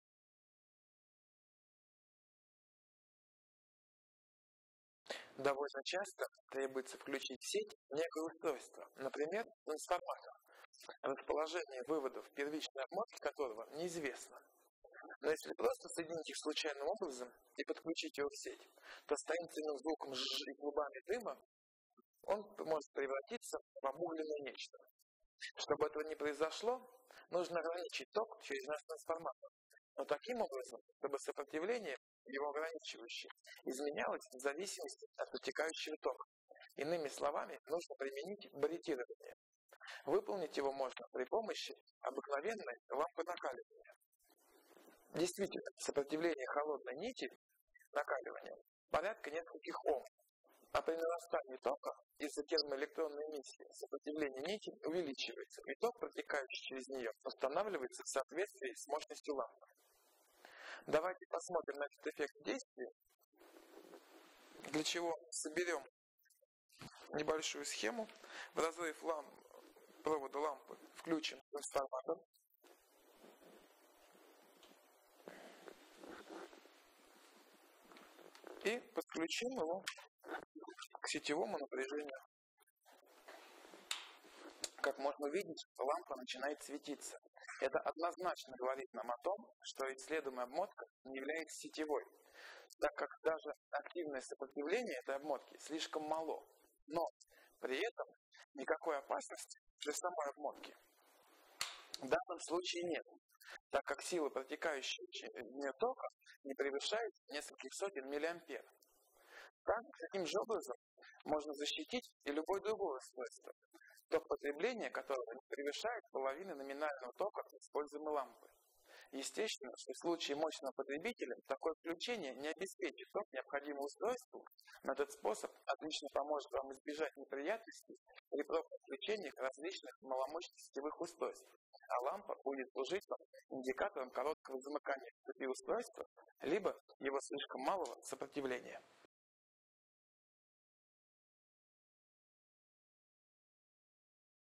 The lamp protection of electronic devices
The lamp protection of electronic devices                  It is often needed to connect a device with the primary bundling outlets unknown to the network. If you simply connect it in a random way and turn it on, you might end up with a "zhhhhhhh" sound, clouds of smoke and as the result the device turns into a charred something.          To avoid this better limit the current with the help of our transformer, but in such a way, that the limiting resistance would vary depending on the current flow. In other words - apply barretting.         It is performed by the means of a simple incandescent lamp. In fact, the resistance of the cold incandescent thread is several Ohms and as the current increases due to the thermoelectric emission the flowing current increases likewise, depending on the lamp capacity.          Here is an example of this effect.          Assemble a small circuit. Take a transformer and connect its bundling to the rupture within the incandescent lamp.          You will see the lamp glowing, which means that the bundling being investigated is not a network one, as the active resistance is too low. HOWEVER, there is no danger for the bundling itself, as the power of the flowing current does not exceed several hundred milliamperes.           In this way any device with the consumption current not exceeding half of the nominal current of the lamp used may be protected.          Naturally, in case of powerful consumption the connection does not secure the needed current, but it helps you avoid problems when first connecting various low-capacity network devices, while the lamp will serve as a short circuit or too low resistance indicator within the device circuit.